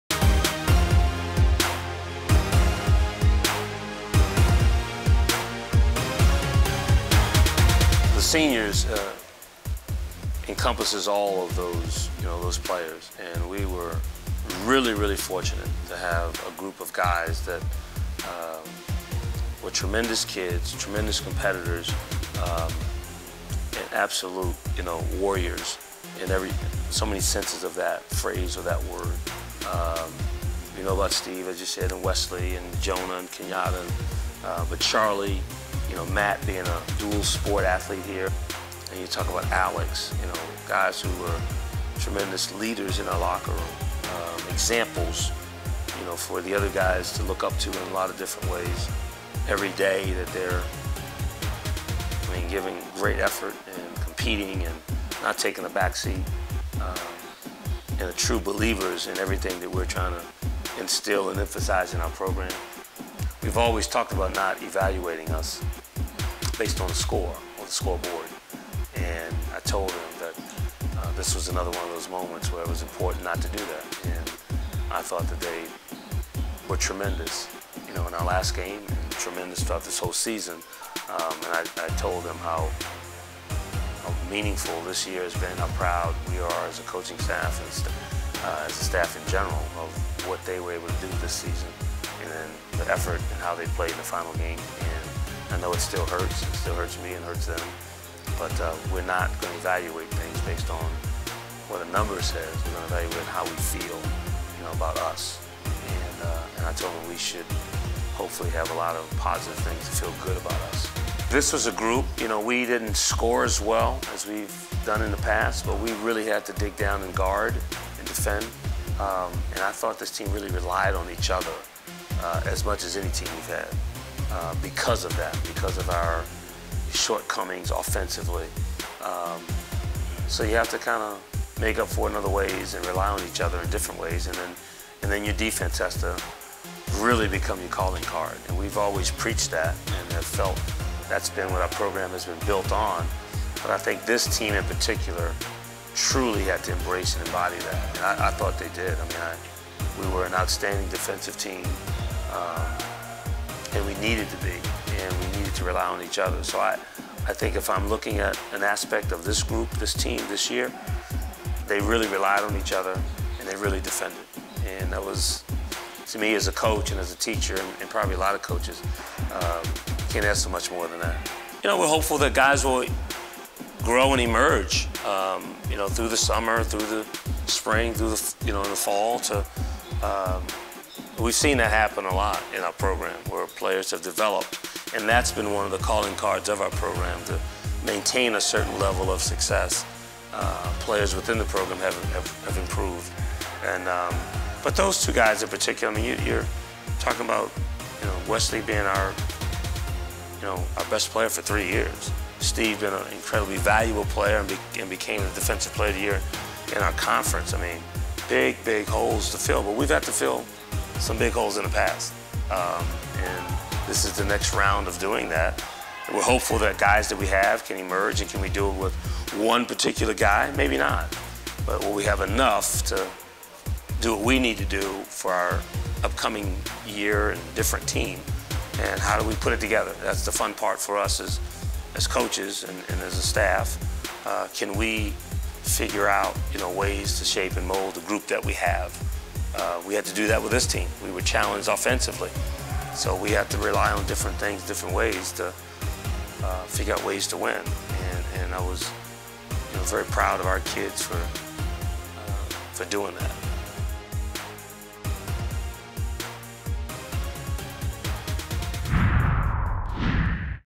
Season Recap: 2014-15 Men's Basketball
Tommy Amaker, The Thomas G. Stemberg '71 Family Endowed Coach for Harvard Men's Basketball, recently sat down with members of the local media to discuss the 2014-15 basketball season.